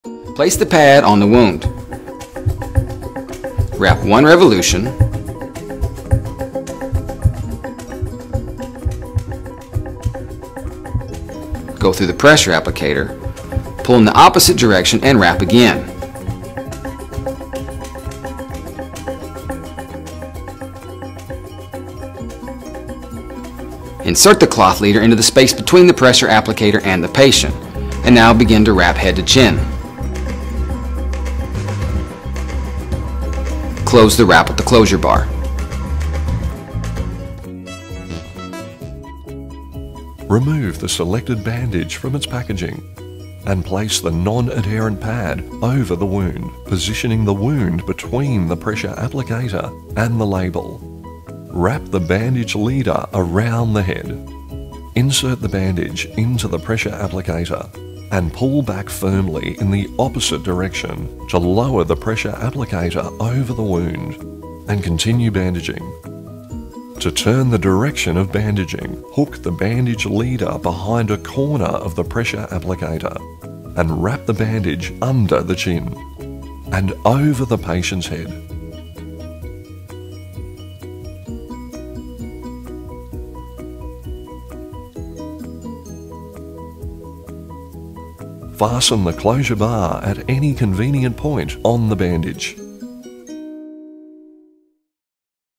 Israeli Bandage For Facial Trauma
Jaw fracture, facial laceration! Israeli Bandage can be used for support and dressing.
Video edited from PerSys Medical & Queensland Ambulance.